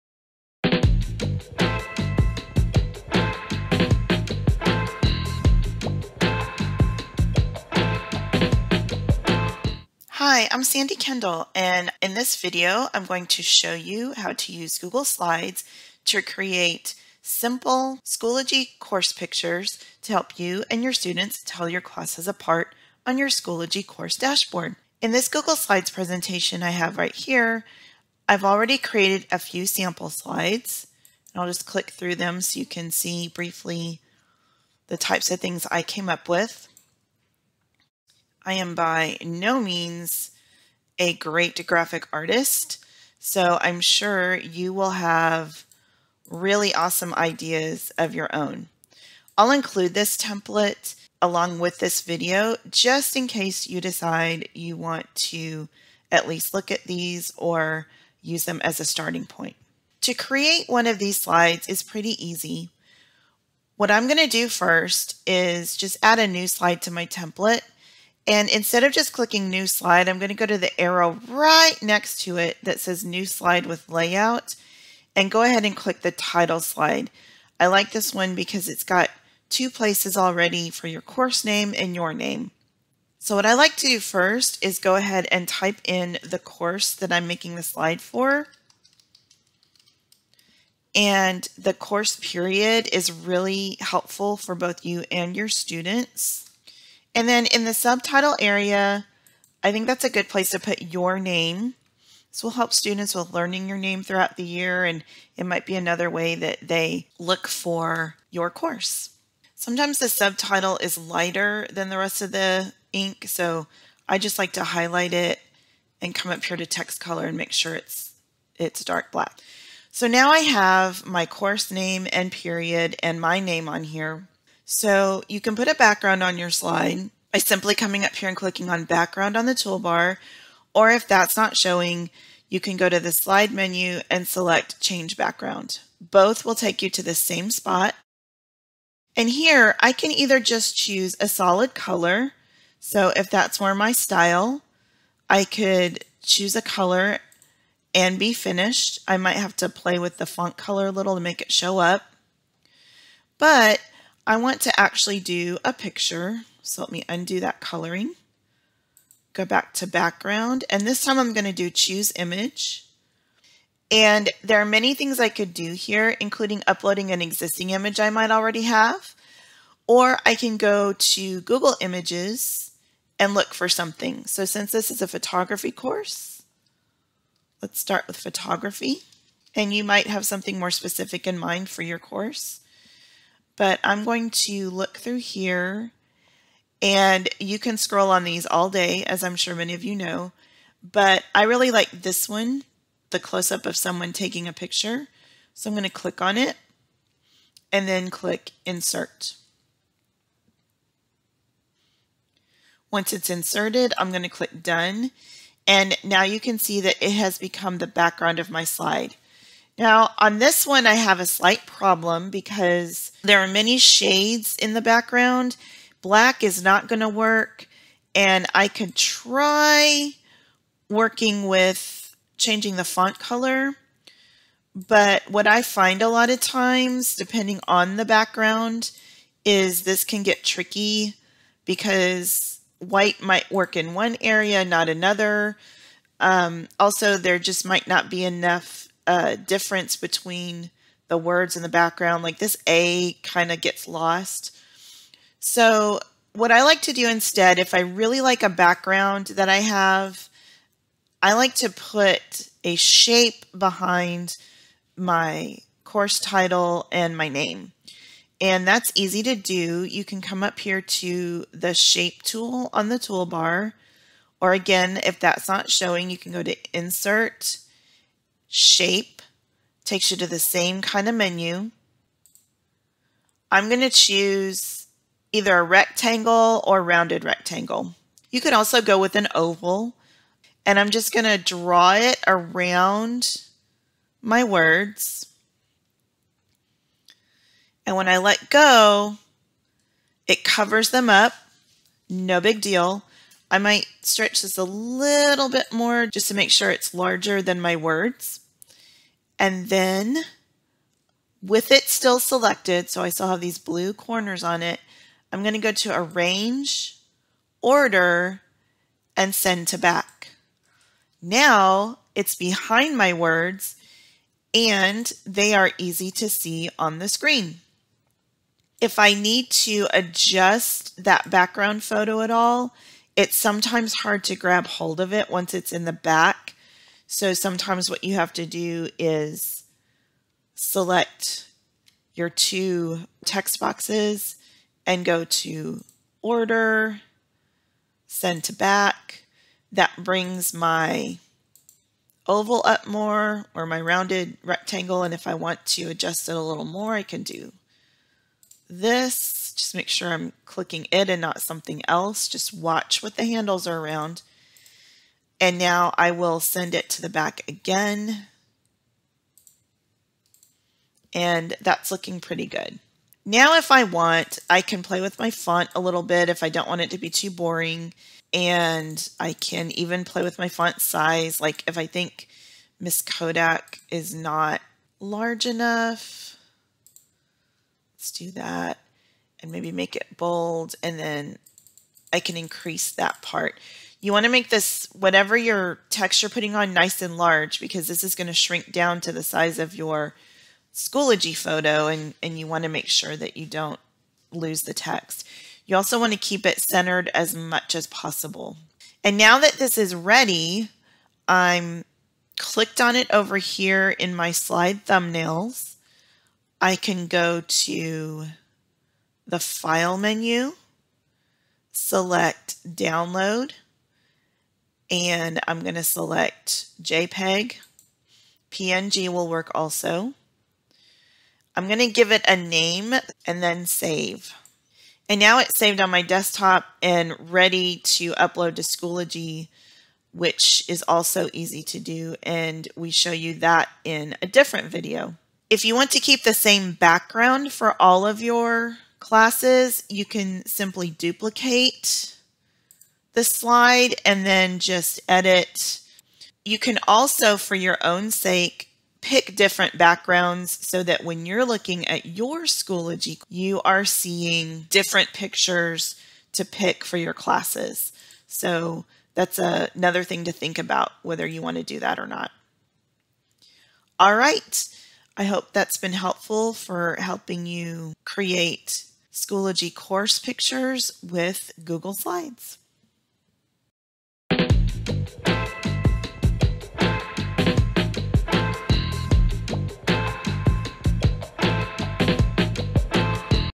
Create Schoology Course Pictures with Google Slides
Organize yourself and make your course stand out on your students' Schoology Course Dashboards with easy-to-make custom course pictures. Created in Google Slides!

Get a Copy of the Google Slides Templates shown in this video